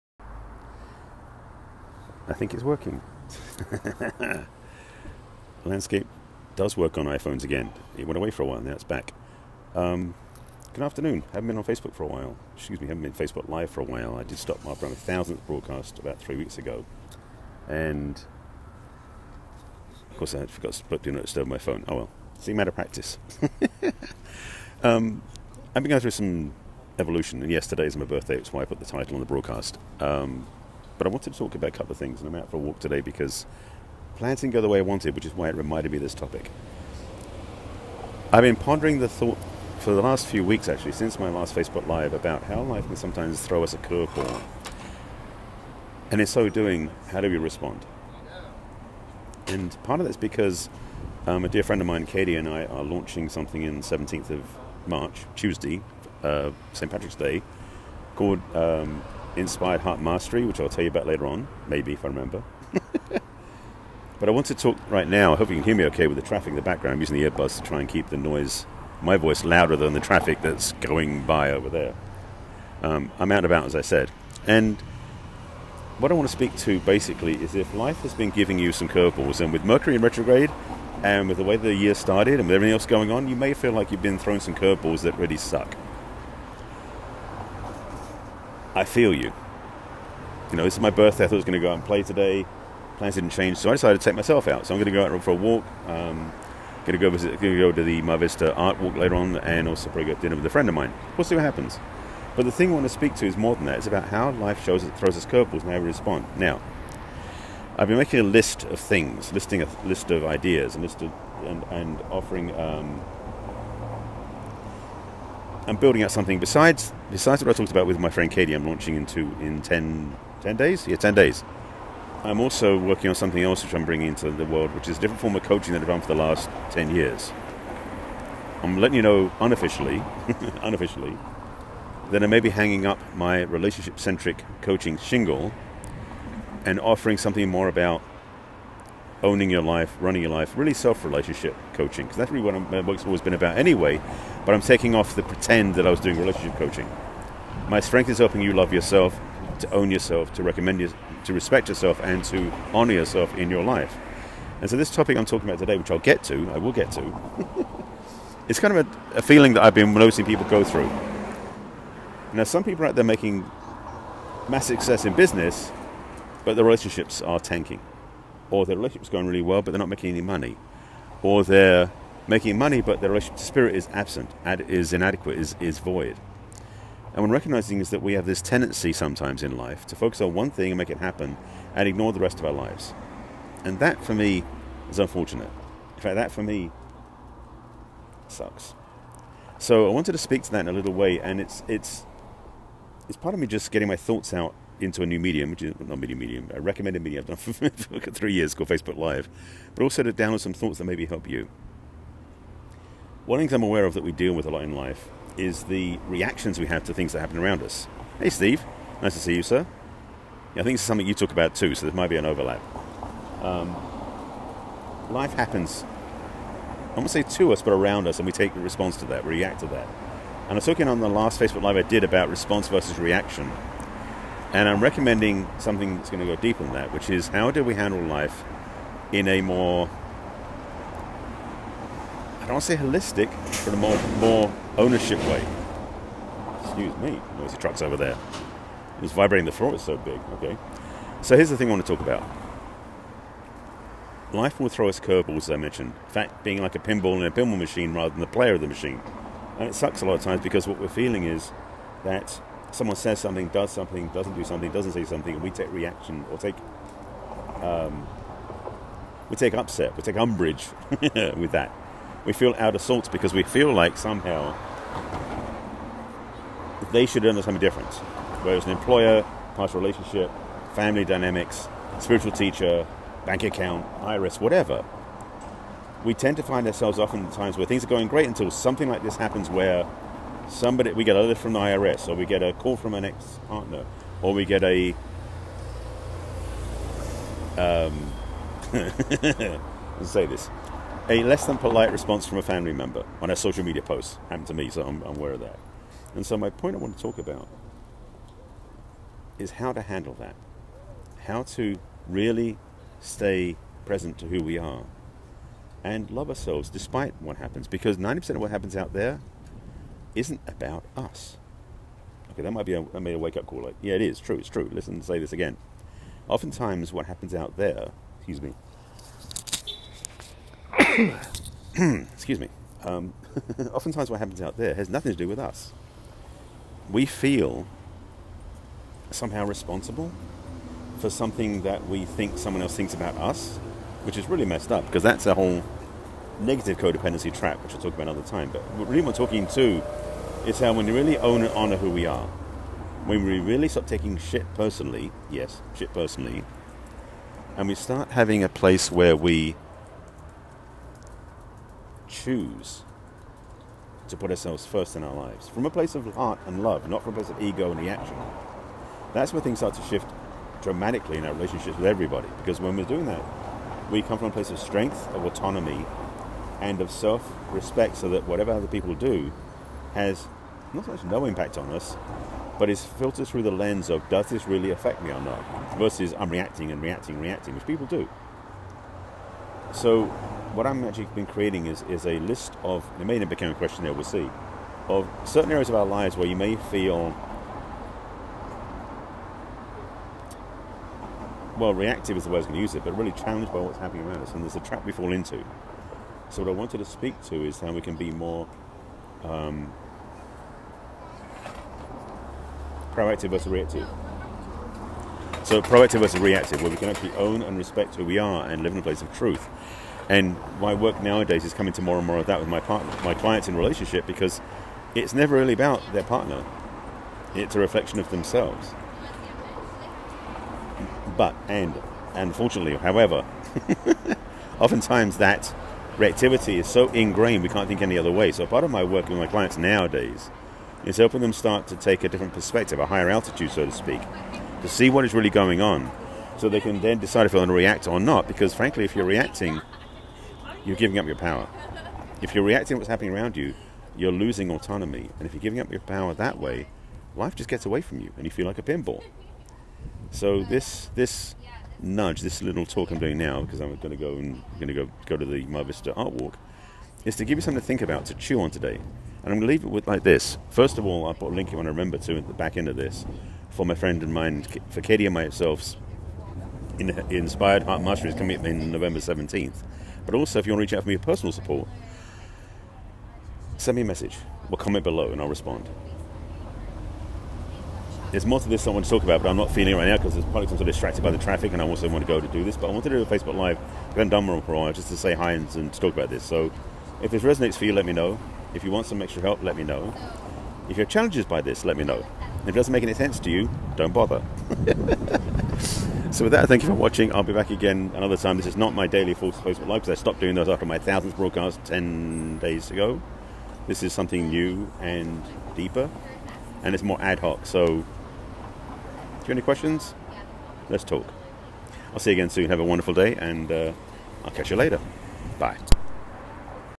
Self-Mastery#001: Feel like a pinball, not a player? When life doesn’t go how you want, what to do?
Self-Mastery#001: Do you feel like the pinball rather than the player? When life doesn’t go how you want, what do you do? A birthday chat!